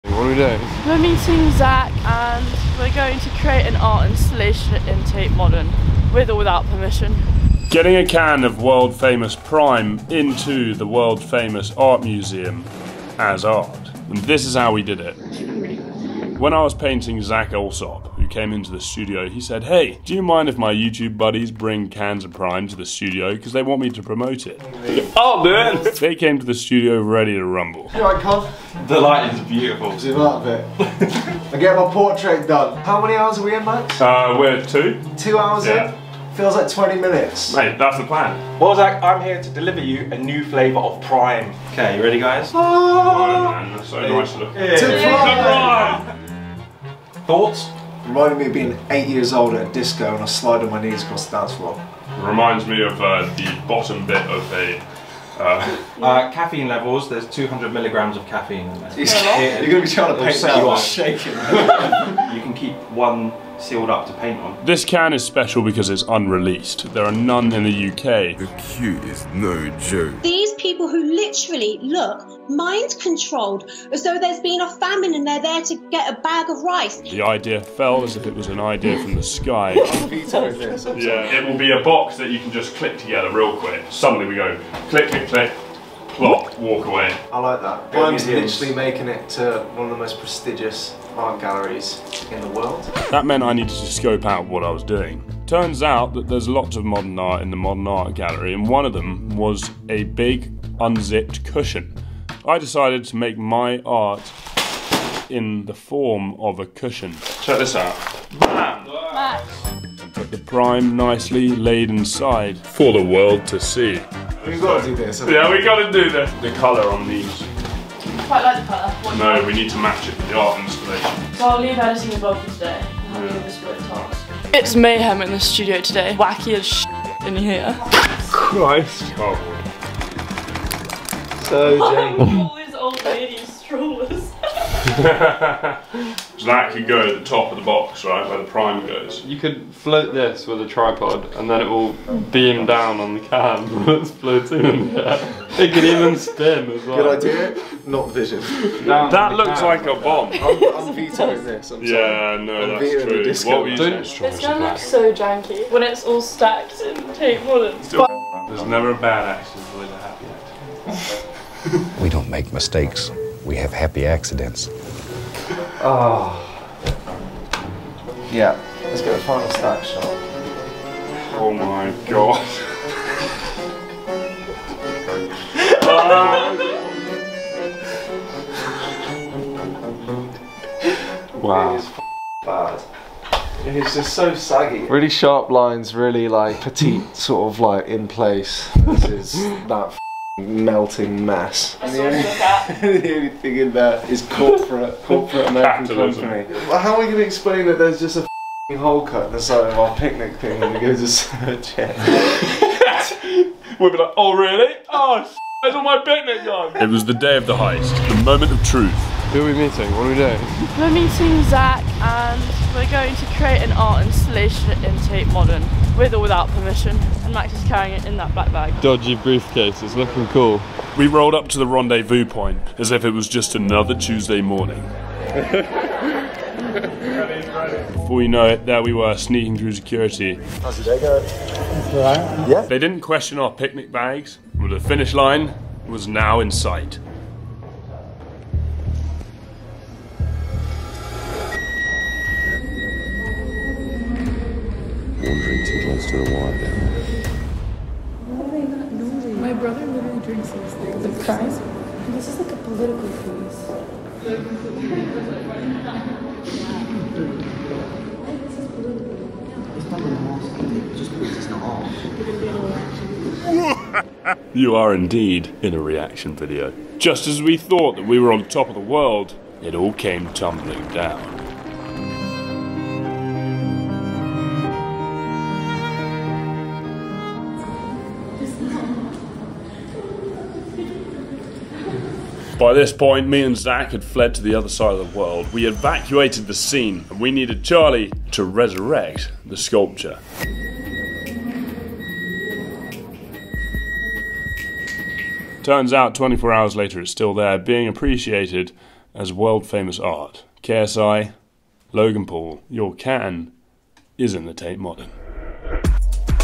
Sneaking $1MILLION PRIME CAN into ART GALLERY! w/ Zac Alsop
Art is subjective. As artists we believe that anything can be Modern Art, even a can of PRIME energy drink. So, we went on a mission to create, sneak in and install an artwork (including a can of PRIME) into one of the most famous art gallery's in the world.

Watch until the end of the video to see if we succeeded.

NEXT VIDEO
Is the "Artwork" still there? And how much is it worth now?

Thank you to @ZacAlsop for helping manifest and execute this crazy plan. Also to @KonFilms for the exclusive can of PRIME.